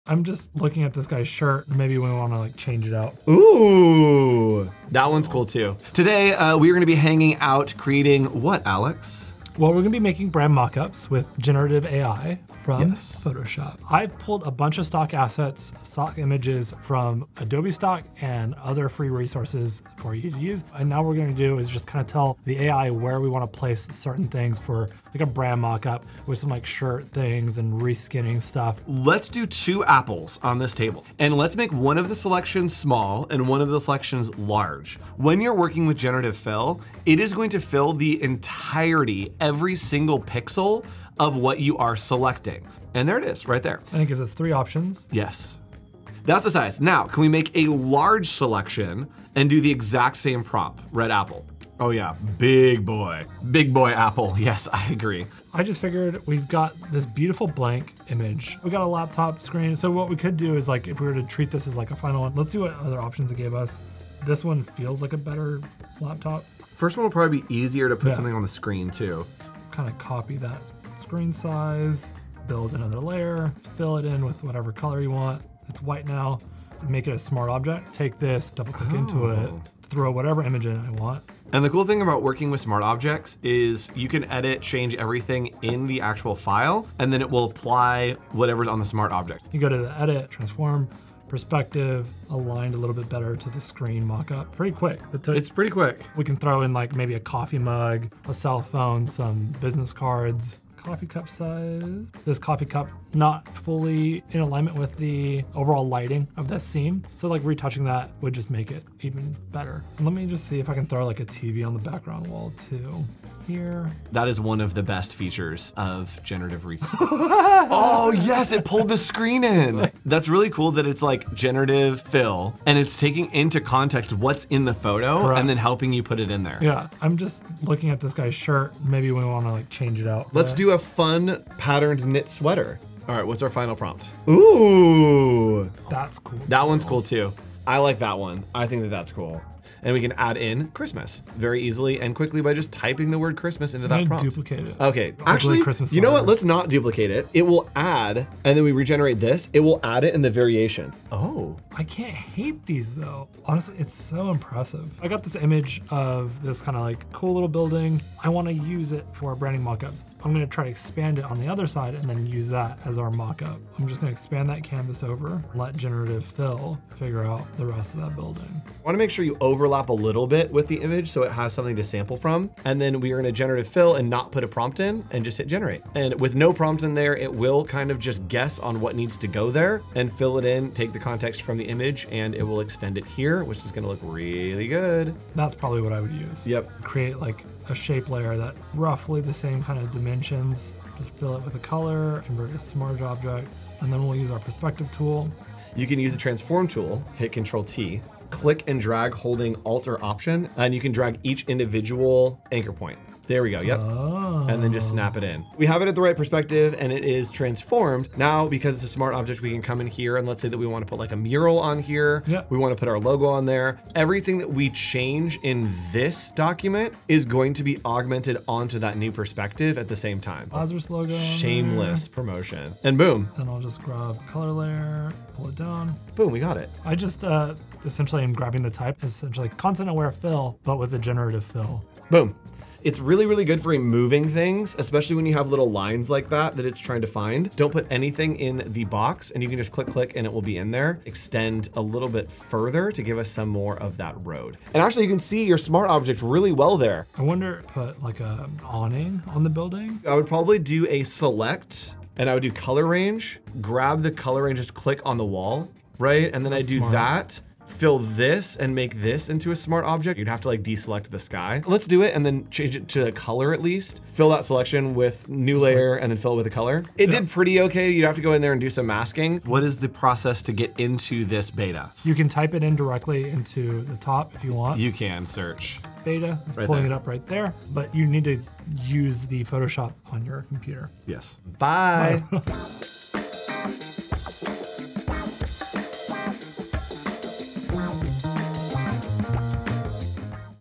Brand Mockups in Photoshop (Beta) with Generative Fill | Tutorial for Beginners | Adobe Photoshop
Learn how to make brand mockups with Generative Fill in Photoshop (beta) in this tutorial with Alex Lazaris and Andrew Hochradel. We will learn how to use generative AI, powered by Adobe Firefly, to combine and scale Stock images for brand mockups.